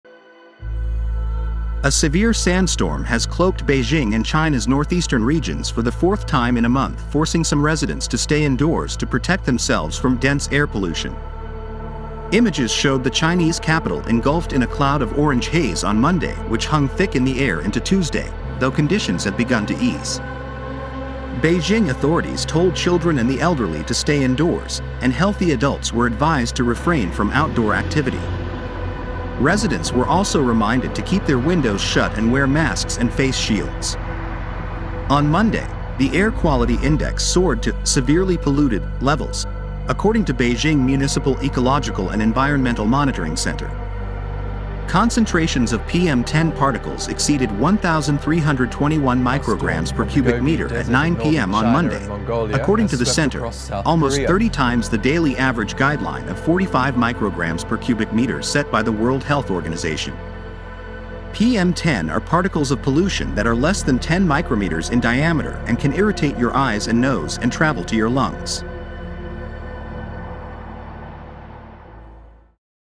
China dust storm
A severe sandstorm has cloaked Beijing and China’s northeastern regions for the fourth time in a month forcing some residents to stay indoors to protect themselves from dense air pollution.

Images showed the Chinese capital engulfed in a cloud of orange haze on Monday which hung thick in the air into Tuesday, though conditions have begun to ease.

Beijing authorities told children and the elderly to stay indoors, and healthy adults were advised to refrain from outdoor activity. Residents were also reminded to keep their windows shut and wear masks and face shields.

On Monday, the air quality index soared to “severely polluted” levels, according to Beijing Municipal Ecological and Environmental Monitoring Center.

Concentrations of PM10 particles exceeded 1,321 micrograms per cubic meter at 9 p.m. on Monday, according to the center – almost 30 times the daily average guideline of 45 micrograms per cubic meter set by the World Health Organization.

PM10 are particles of pollution that are less than 10 micrometers in diameter and can irritate your eyes and nose and travel to your lungs.